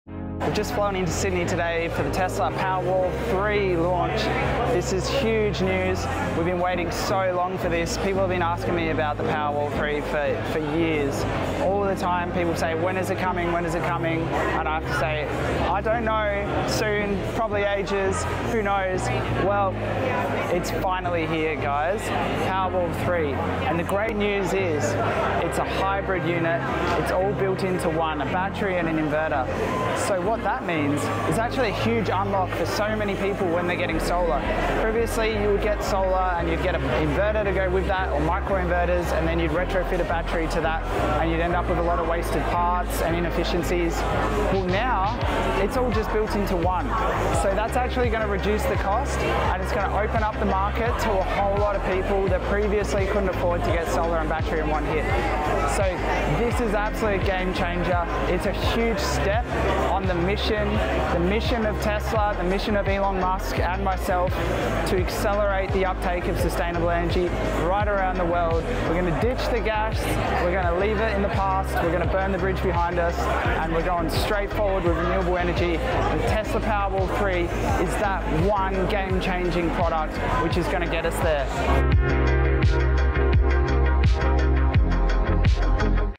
Tesla Powerwall 3 Launch Event Sydney Australia.
Luke from Lightning Energy - Melbourne Australia goes to the Tesla Powerwall 3 launch event in Sydney Australia to see what all the hype is about.